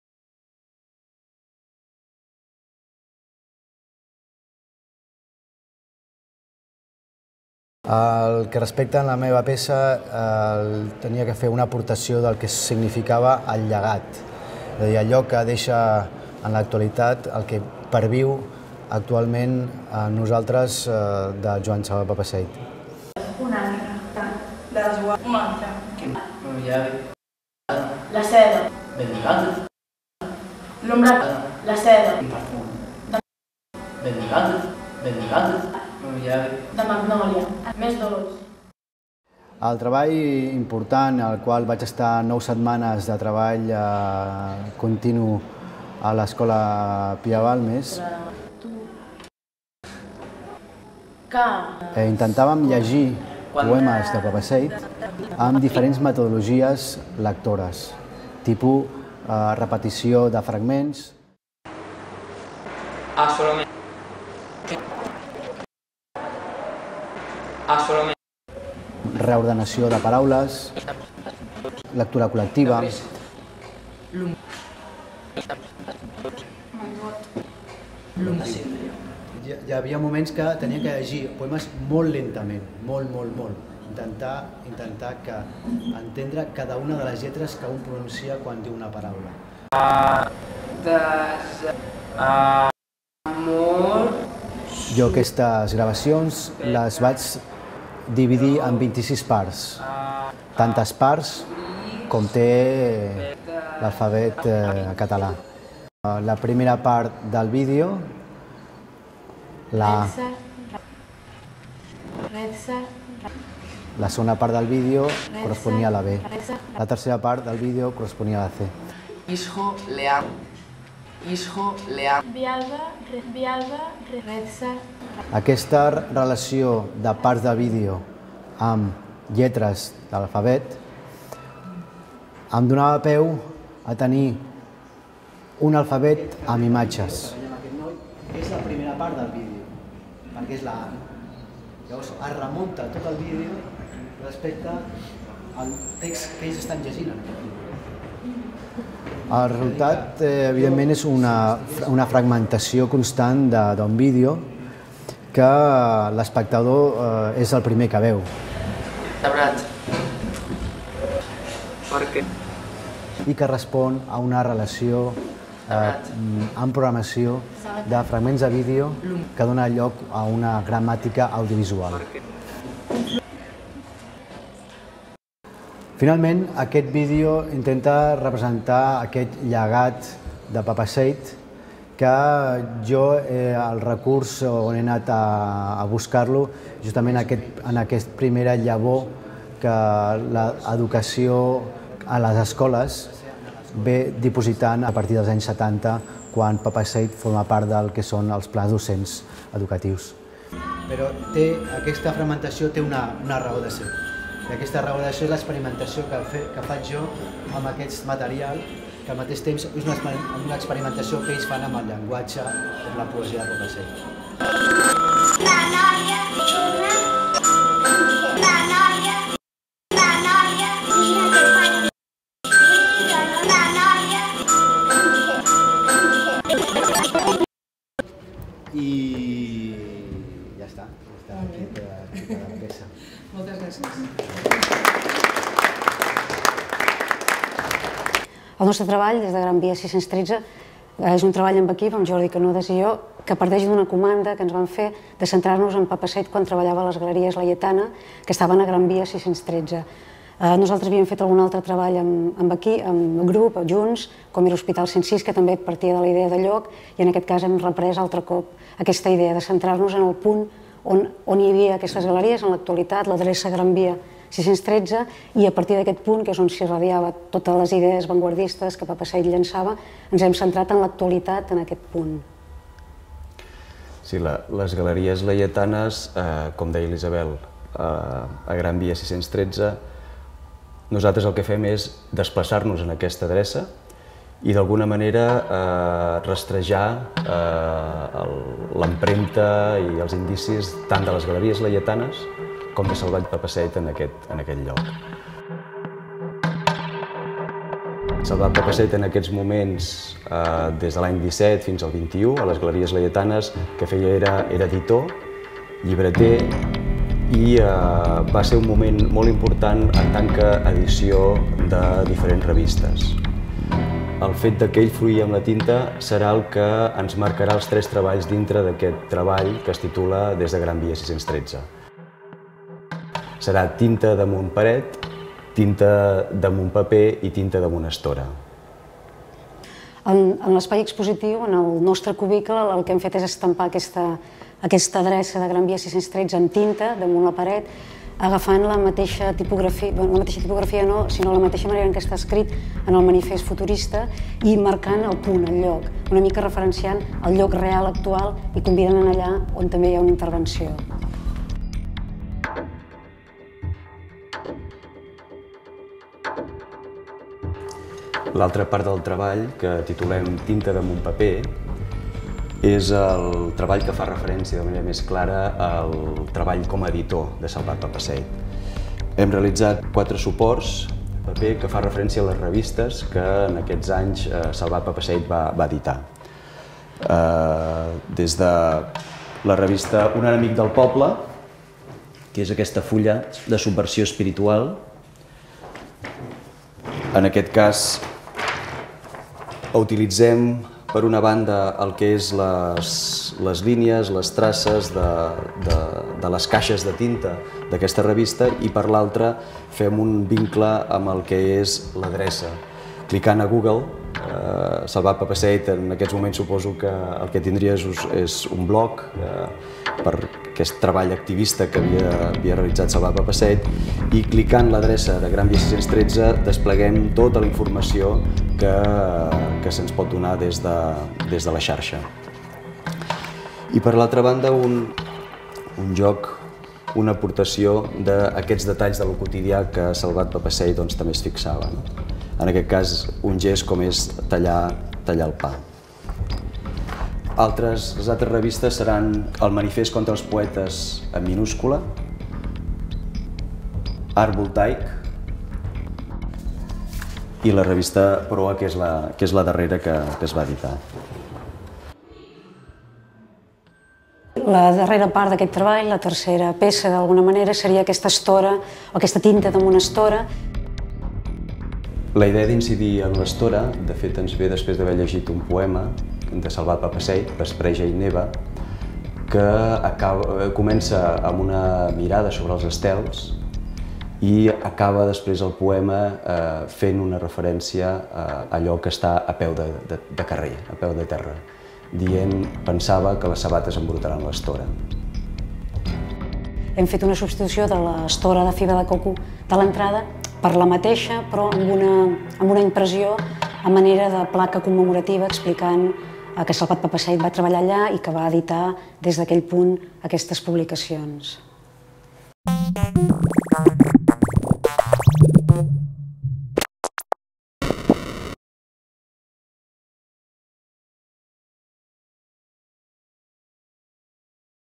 SALVAT-PAPASSEIT poetavanguardistacatalà (Intervencions d'artistes)
23 desembre 2010 - 3 abril 2011
Diferents artistes han realitzat un projecte específic per l'exposició posant de relleu la vigència i la creativitat del poeta i la seva escriptura ètica.

Domènec  i Eloi Puig  (ANJOPA SEITPA-VATSAL)
Isabel Banal - Jordi Canudas (GRAN VIA, 613 - BARCELONA)

Comissariat: Pilar Bonet i Maia Creus
Organitza: Arts Santa Mònica - Institució de les Lletres Catalanes
Col·labora: Edicions 62